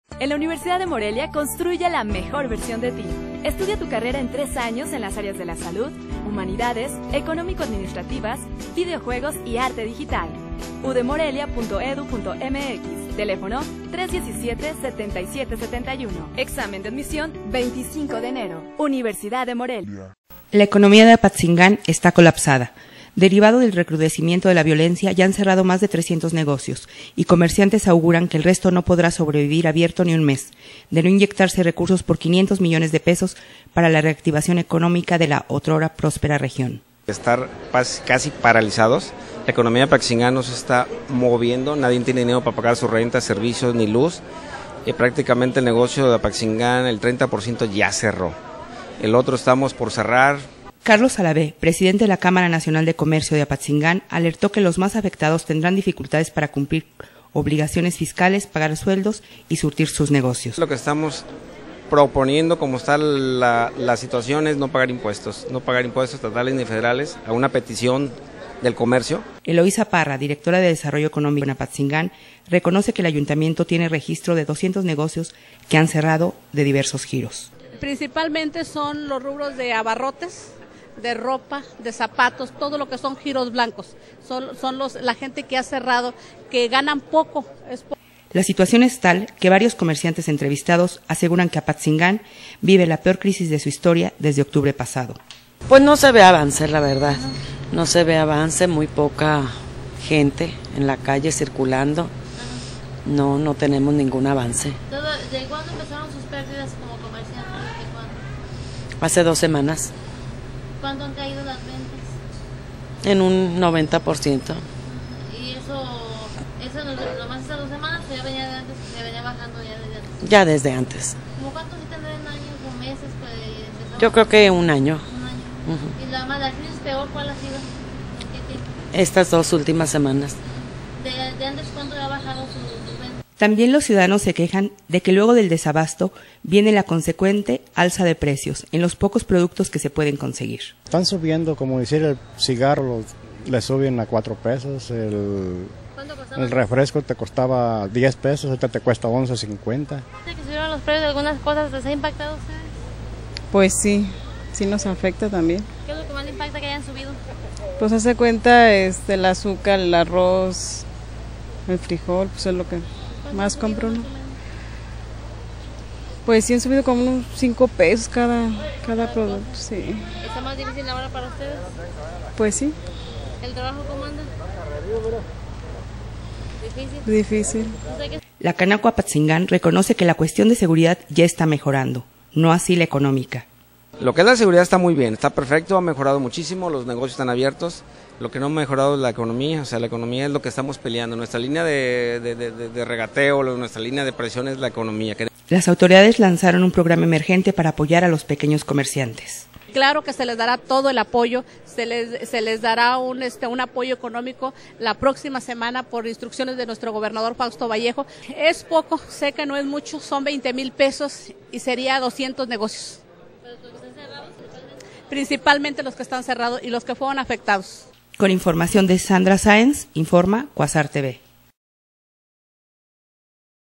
Colapsa economía en Apatzingán, cierran 300 negocios
La economía de Apatzingán está colapsada. Derivado del recrudecimiento de la violencia ya han cerrado más de 300 negocios y comerciantes auguran que el resto no podrán sobrevivir abiertos ni un mes, de no inyectarse recursos por 500 millones de pesos para la reactivación económica de la otrora próspera región...